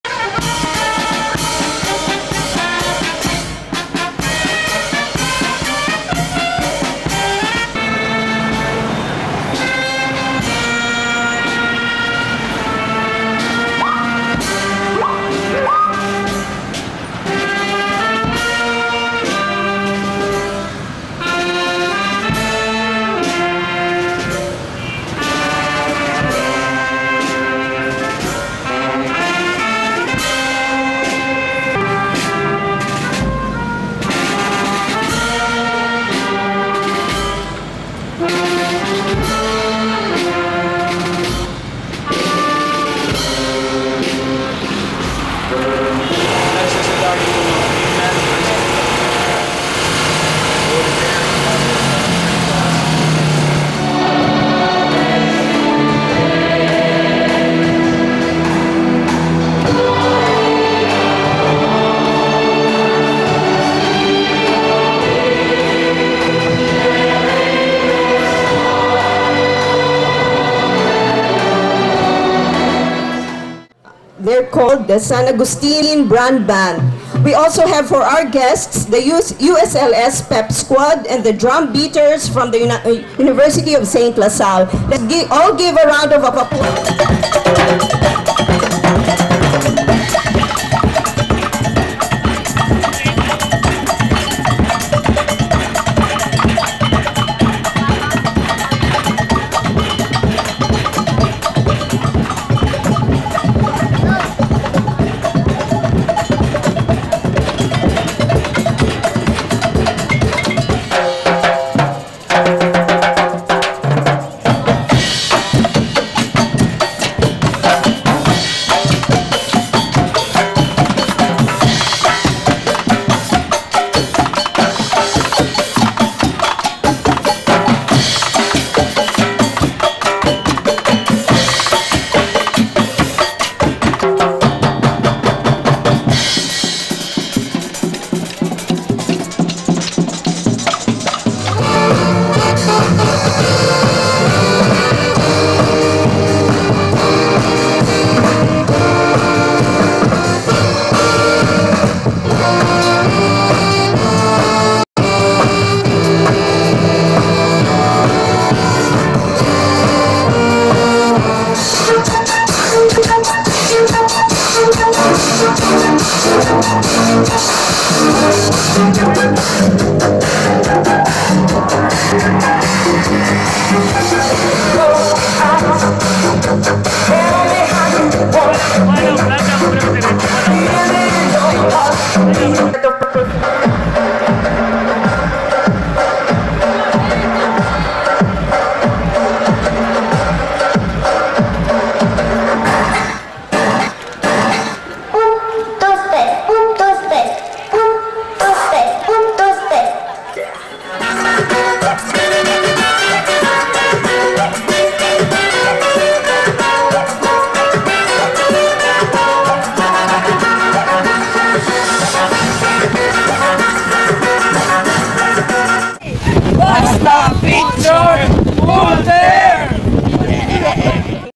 OLPH Shrine Fiesta
The USLS-IS Drumbeaters and the USLS Pep Squad joined the celebration of the feast of the Diocesan Shrine of Our Lady of Perpetual Help last June 20, 2010.  The said event was also participated by other guest from other institutions.  The celebration started with a procession and a mass followed by a showcasing of talents in the evening.